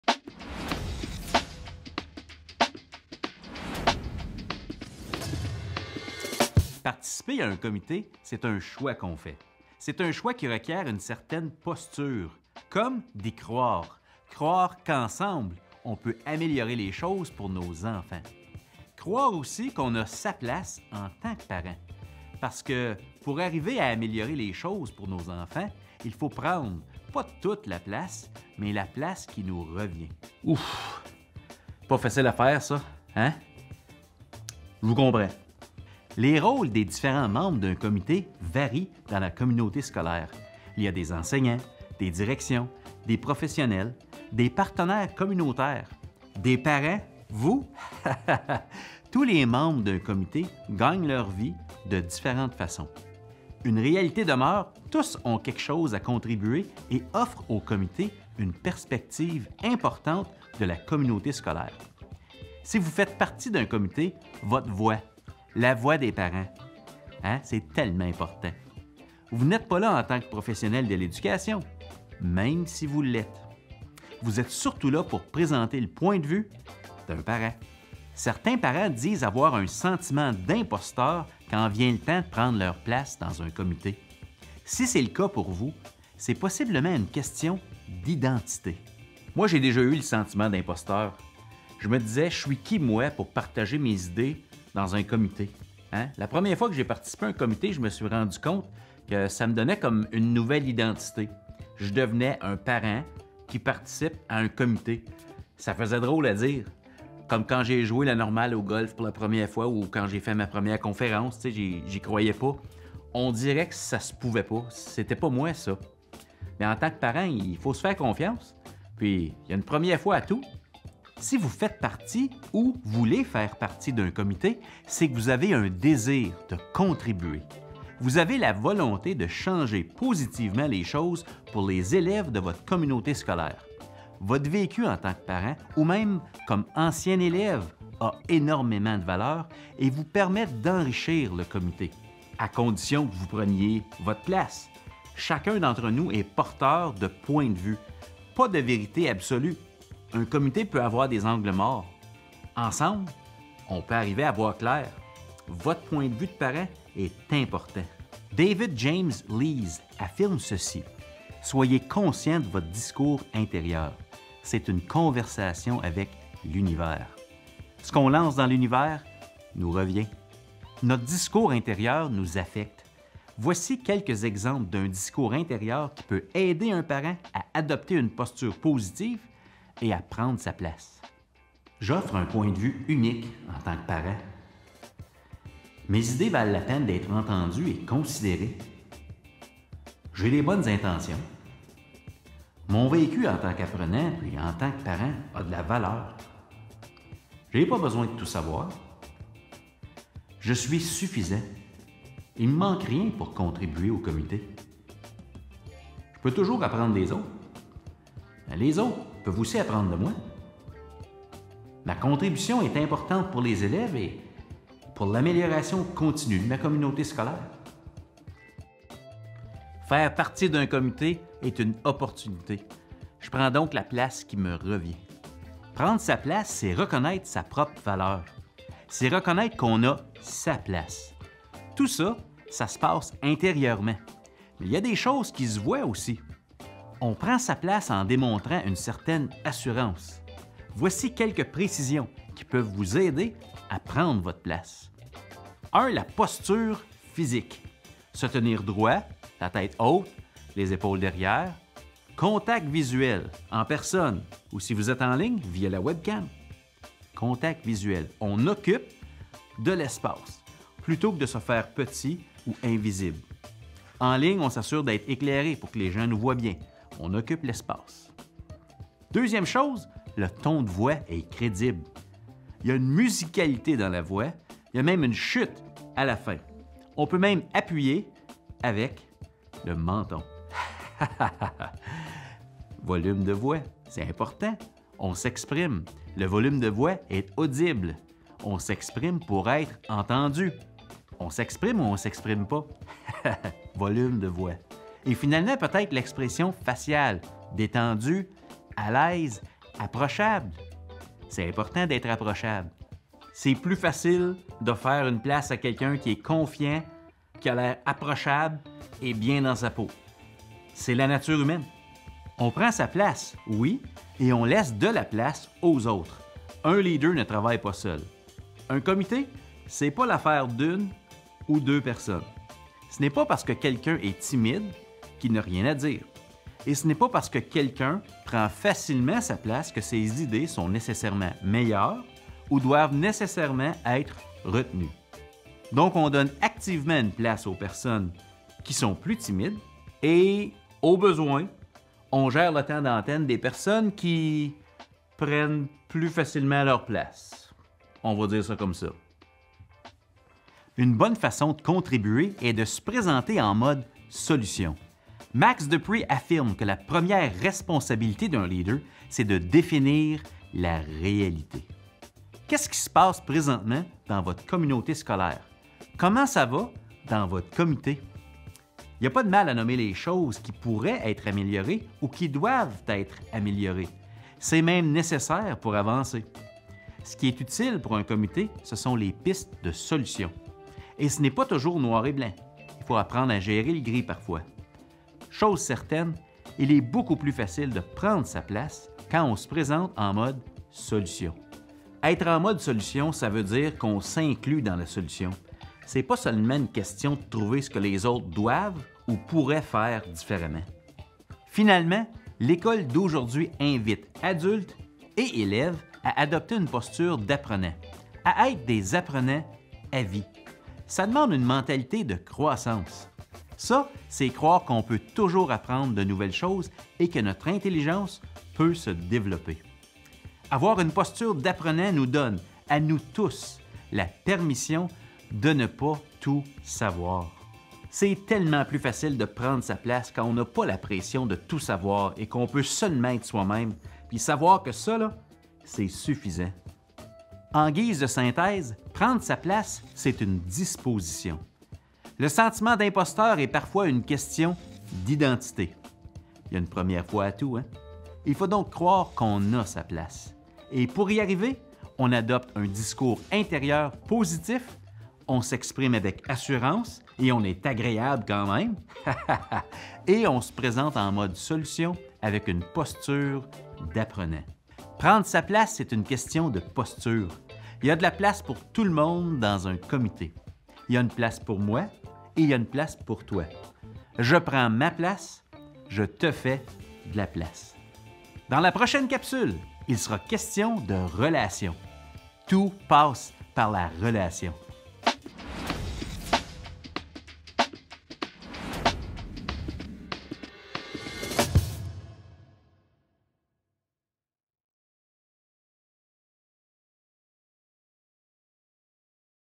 Je prends ma place - Capsule #2
Les capsules « Tonif pour parents engagés » sont des séances de coaching pour les parents nouvellement engagés ou déjà actifs dans des comités scolaires.

Animées par Marius Bourgeoys de chez EscouadeÉDU, les capsules traitent de façon dynamique des attitudes et des compétences clés comme le leadership, la posture d’apprenant, les relations interpersonnelles, le travail d’équipe et la communication, et ce, en vue d’outiller et d’aider les parents engagés à apporter une contribution positive et constructive dans les divers comités auxquels ils siègent.

Ce projet est financé grâce à une subvention conjointe des gouvernements du Québec et de l’Ontario.